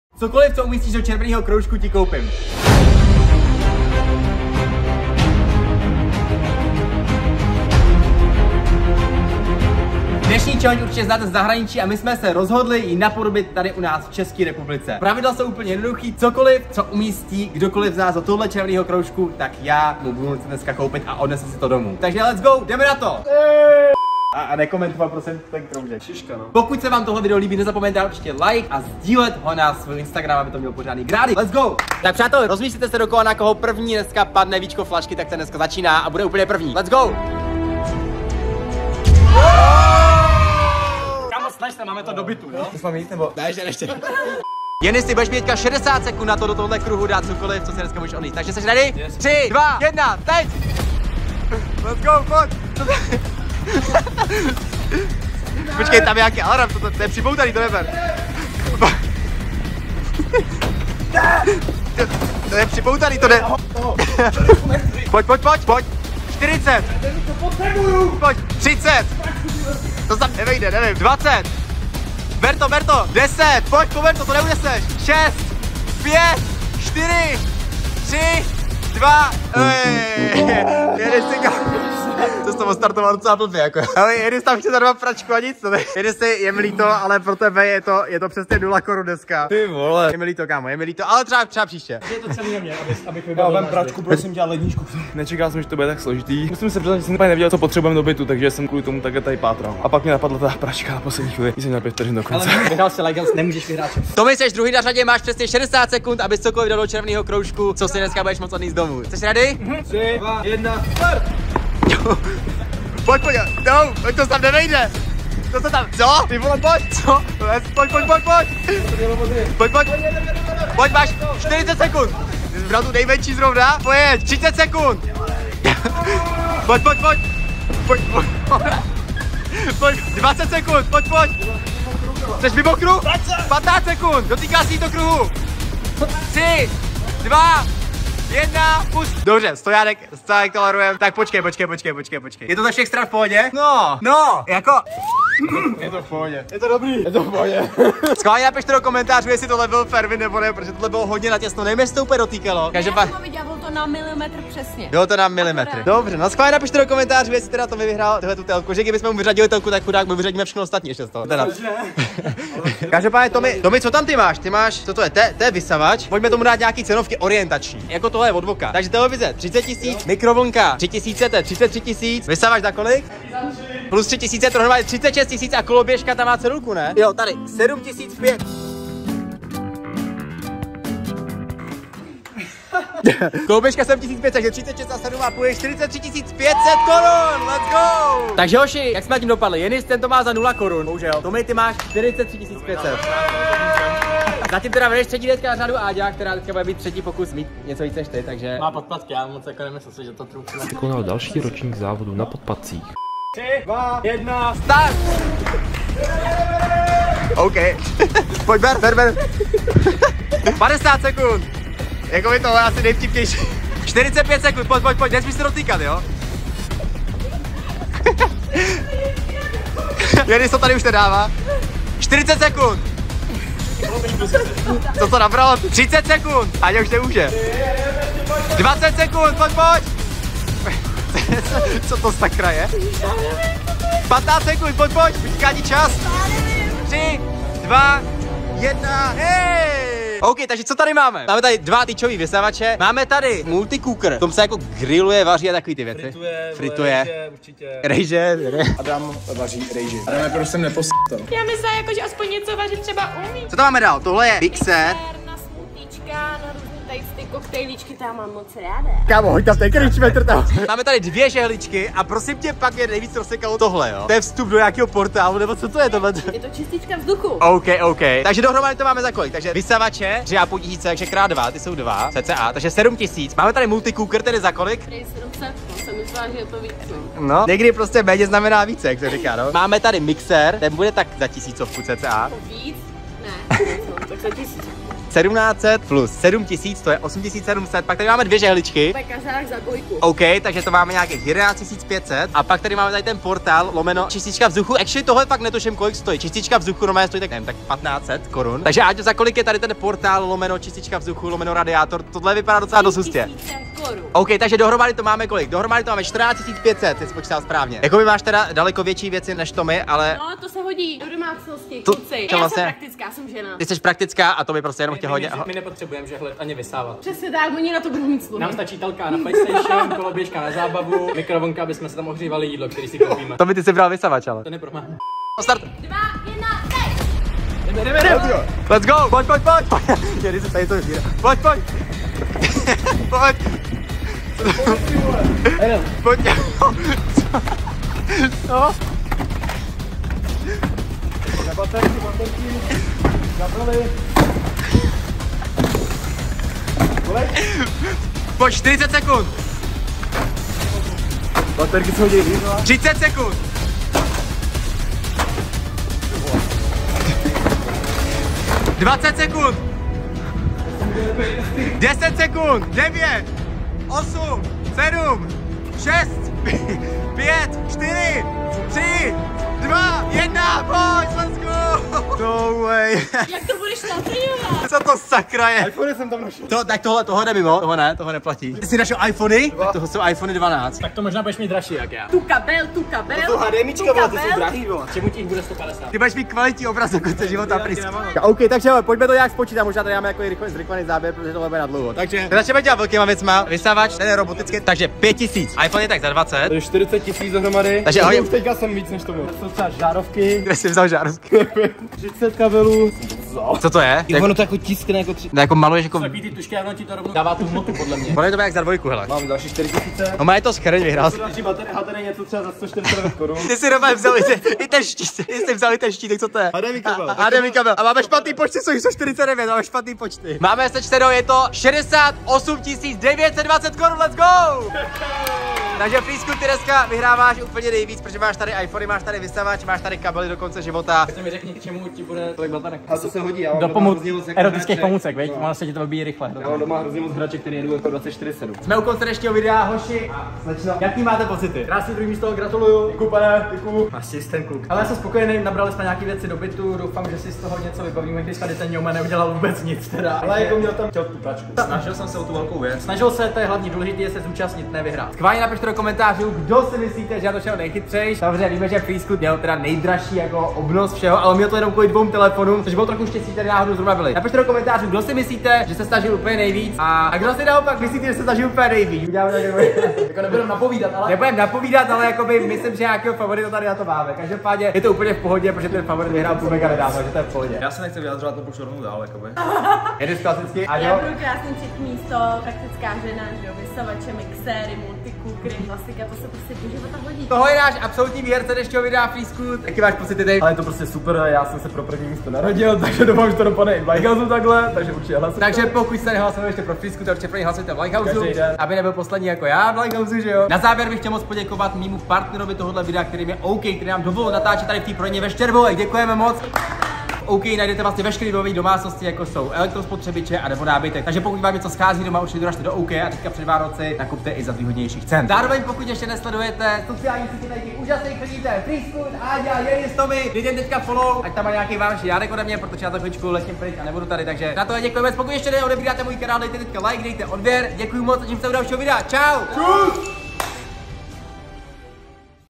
VŠE, CO UMÍSTÍŠ DO ČERVENÉHO KROUŽKU TI KOUPÍM - 60 SEKUND CHALLENGE /w Tomi, Jenis, Freescoot, Áďa
Nepiš hate komentáře, buď raději ALRIGHT!
Přemýšlej nad tím, jak si splnit své sny, namísto toho,  jak zničit sny ostatním. Díky. Peace.

❤️ Mockrát děkuju za všechnu podporu!